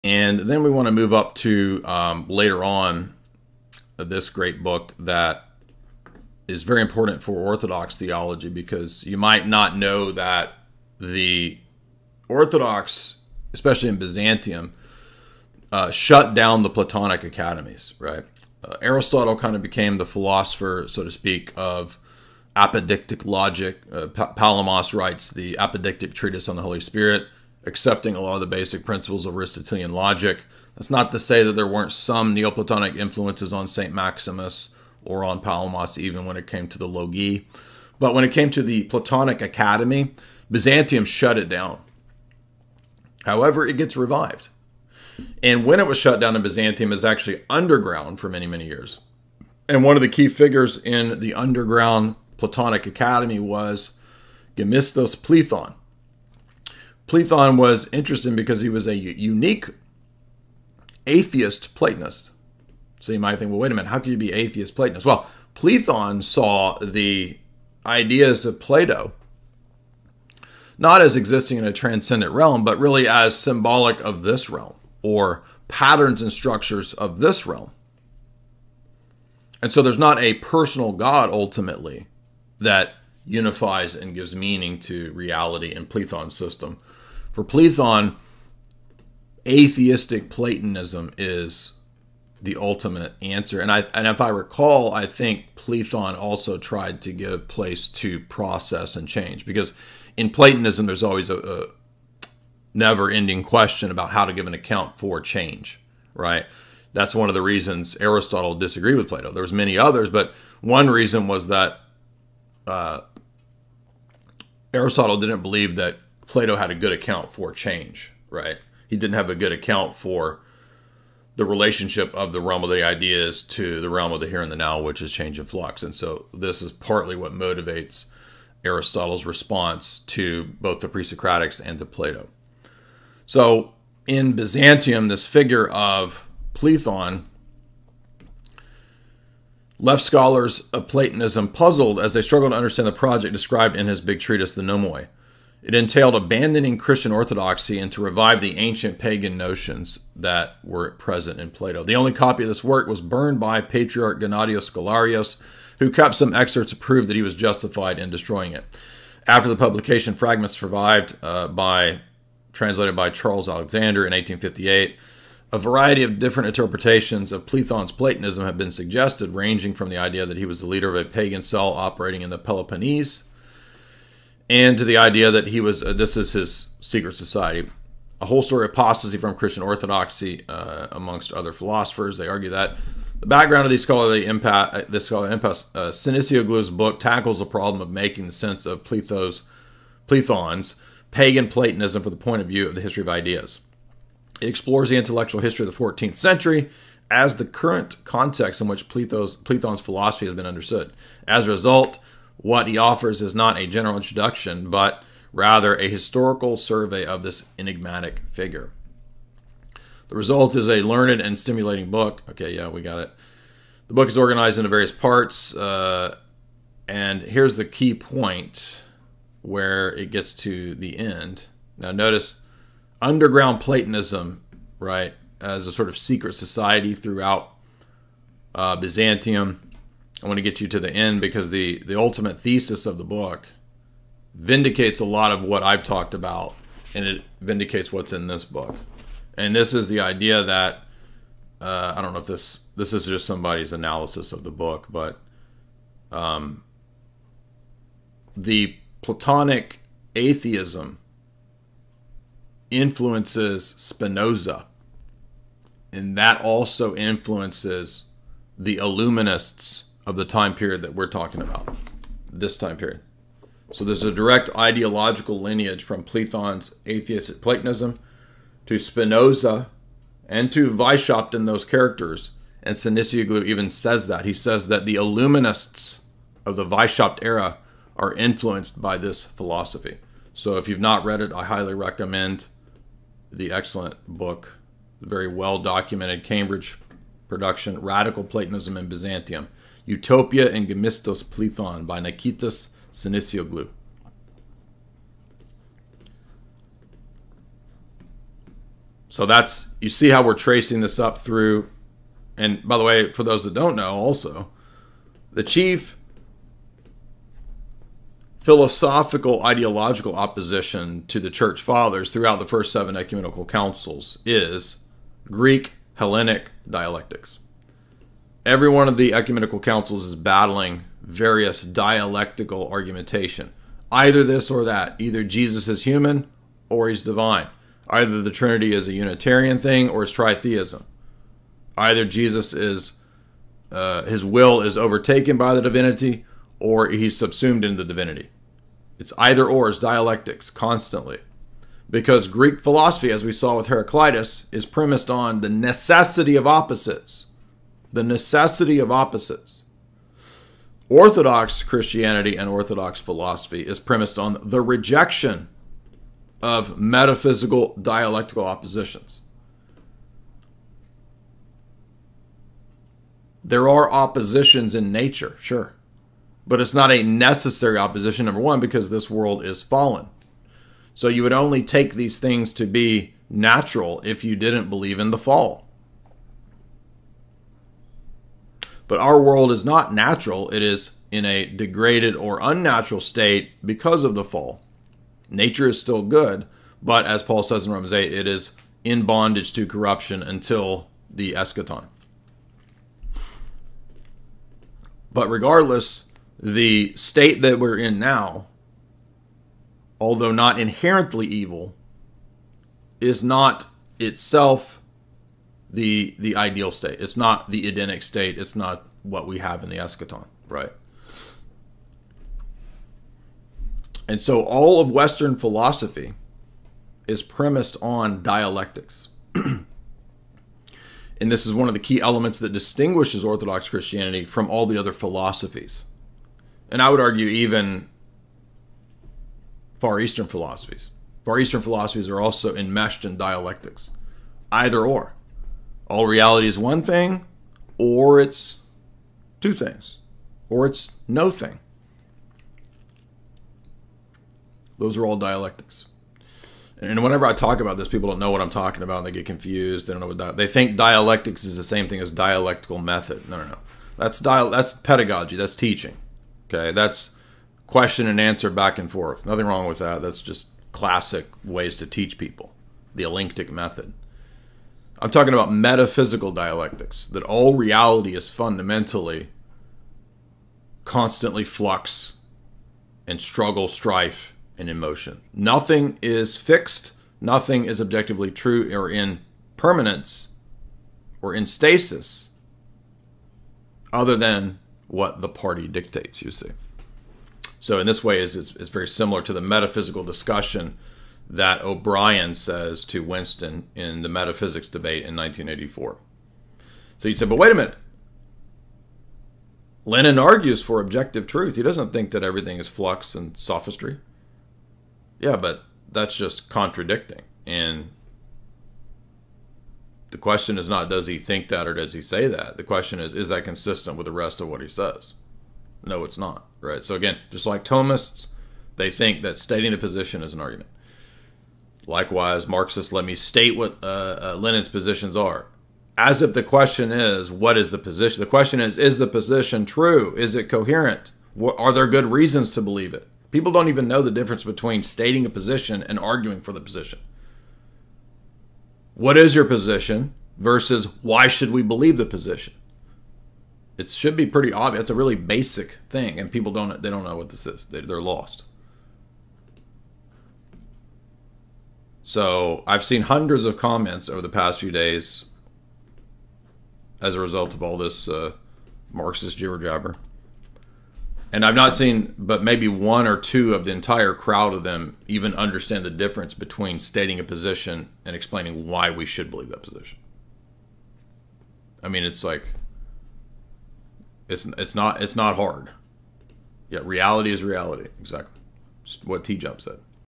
Plato | Plethon | Spinoza | Illuminati | Marxism | Modern Hellscape | 1984 ft. Jay Dyer
@JayDyer discusses about Gemistos Plethon and his underground neo-platonism during Byzantium and his work which was burned by Pope Gennadios since it was the aim to replace Orthodox Christianity in Byzantium with paganism which was radical in  nature so giving rise to atheistic Platonism and it's later effect on Renaissance scholars like Spinoza whose theory further influenced the illuminists like Adam Weishaupt and later systems like Marxism and which is also discussed in 1984 between O'Brian and Winston.